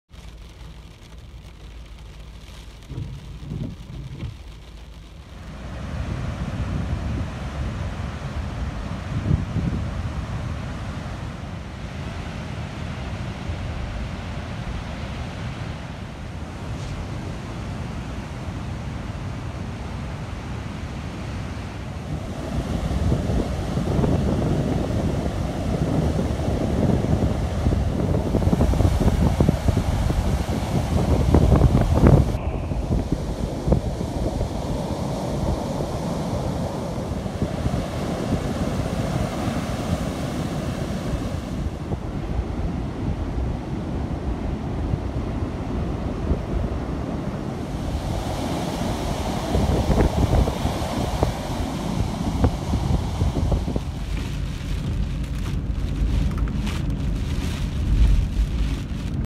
Daily Outer Banks Getaway - November 15, 2018
Video documenting the scenic beauty of NC's Outer Banks on November 15, 2018.